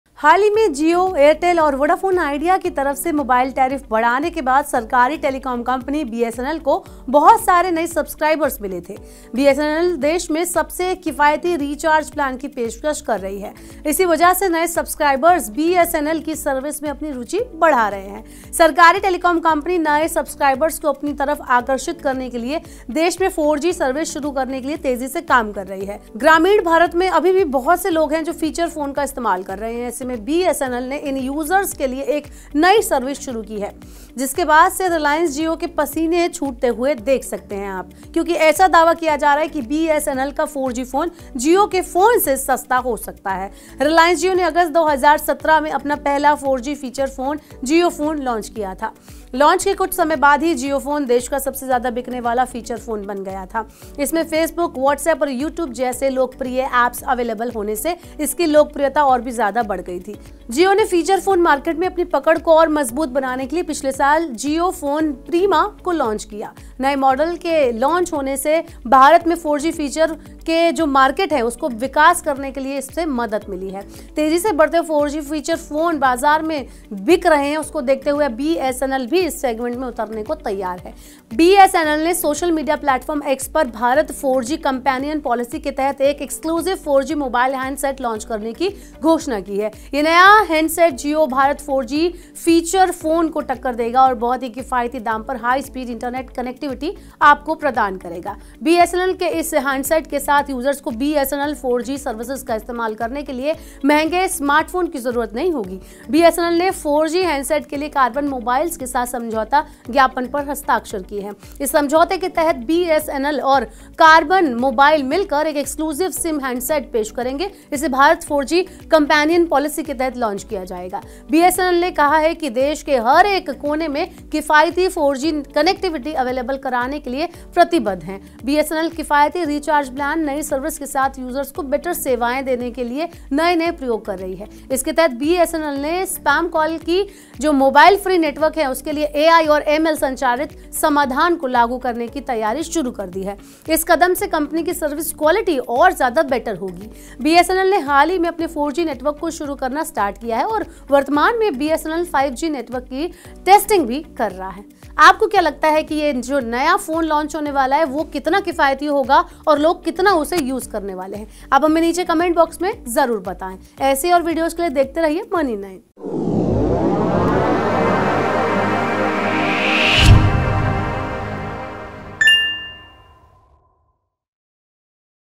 BSNL | Jio Bharat 4G फोन से सस्‍ता होगा BSNL 4G मोबाइल!
भारत संचार निगम लिमिटेड यानी BSNLने सस्‍ते मोबाइल रिचार्ज प्‍लान के साथ ही. अब सस्‍ता 4जी मोबाइल फोन भी लॉन्‍च करने की घोषणा की है... BSNL किस कंपनी के साथ मिलकर बनाएगी 4G फोन? इस फोन में क्‍या-क्‍या होंगे फीचर्स? Jio के 4G फोन से कितना अलग होगा BSNL का ये नया फोन? जानने के लिए देखिए Money9 का यह वीडियो।